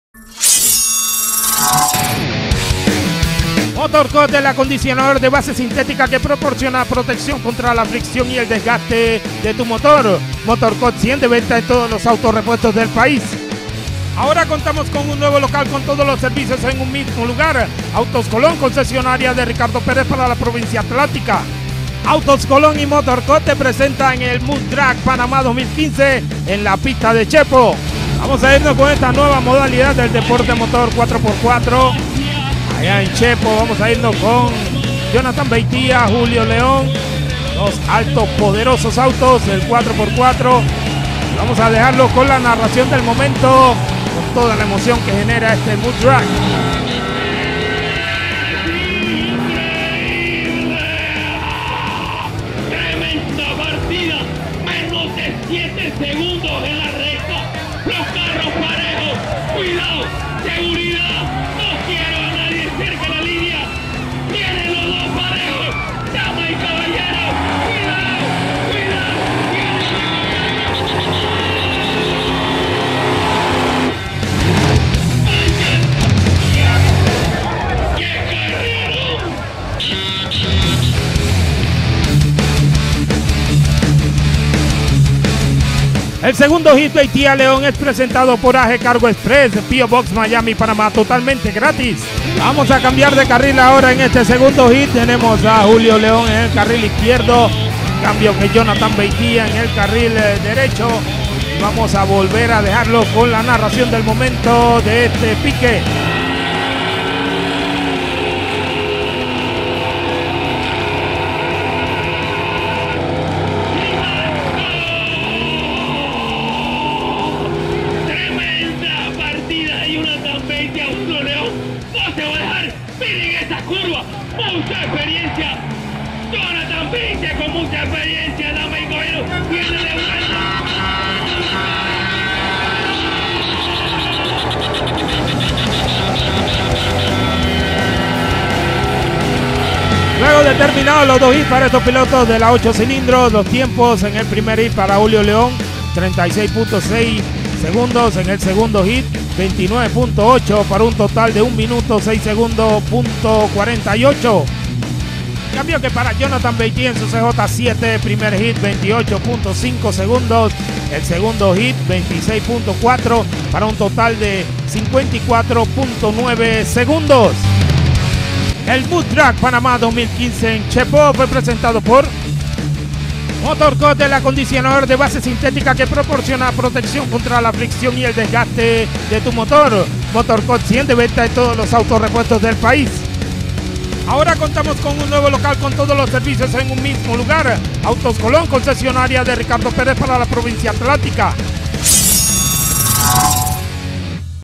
MUD-DRAGS PANAMÁ 2015 - Jonathan Beitia vs Julio León
Nueva modalidad de competencias de 4X4 en Panamá que contó con la presencia de gran cantidad de fanáticos de este deporte a motor.
Te presentamos 2 competidores de alto voltage en la categoría de los 8 cilindros Jonathan Beitia vs Julio León.